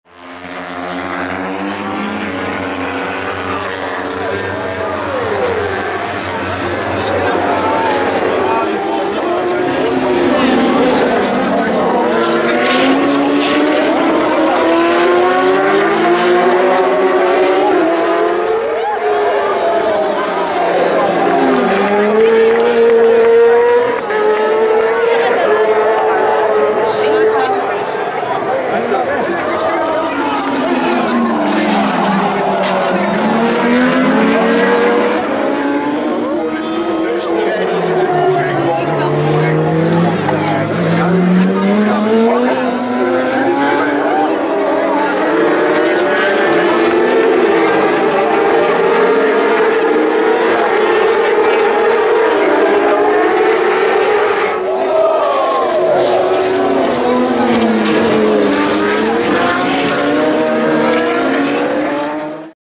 British Superbike (bsb) Assen 2014 Crashes+Action
Some action in the geert timmer bocht during the British superbikes.

Nice event with many different motorcycles
Dutch Superbikes
Dutch Supersports
British Supersports
& more!